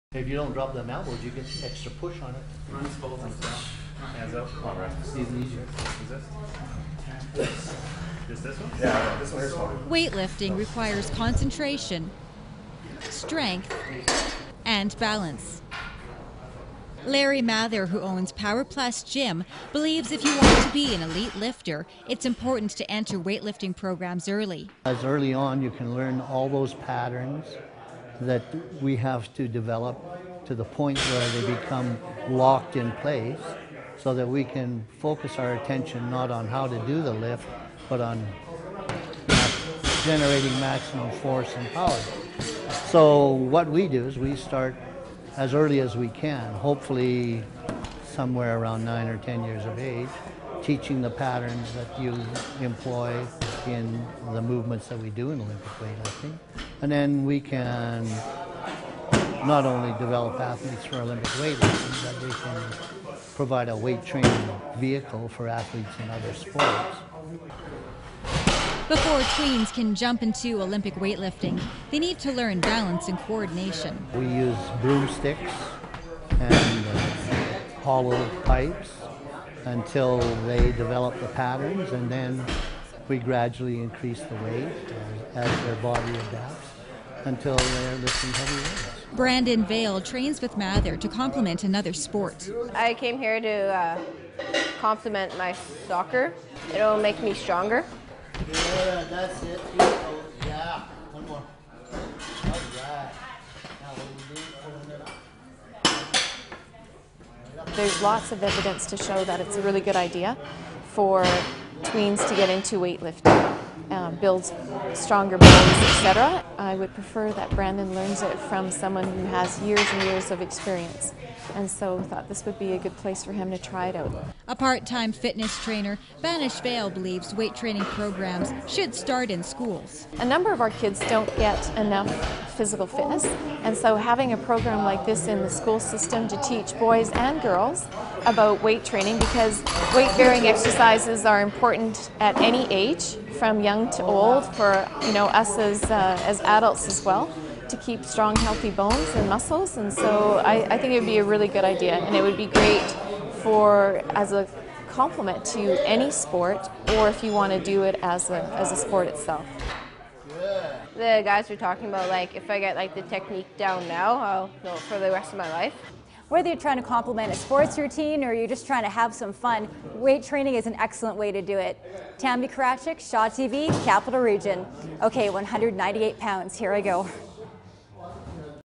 Weightlifting Teens
Shaw TV Edmonton reporter Tammy Karatchuk spoke with a local weight trainer about the benefits of weightlifting at a young age

Video courtesy: ShawTVEdmonton